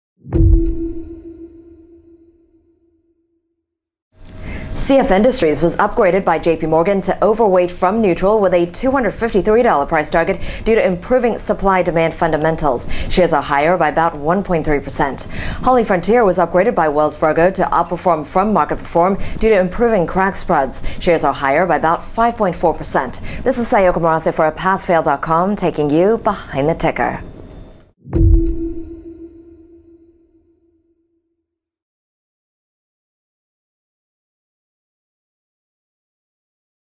Passfail.com News: Analyst Moves: CF, HFC
CF Industries (CF) was upgraded by JP Morgan (JPM) to overweight from neutral with a $253 price target due to improving supply/demand fundamentals.

Shares are higher by about 1.3 percent.

HollyFrontier (HFC) was upgraded by Wells Fargo (WFC) to outperform from market perform due to improving crack spreads.

Shares are higher by about 5.4 percent.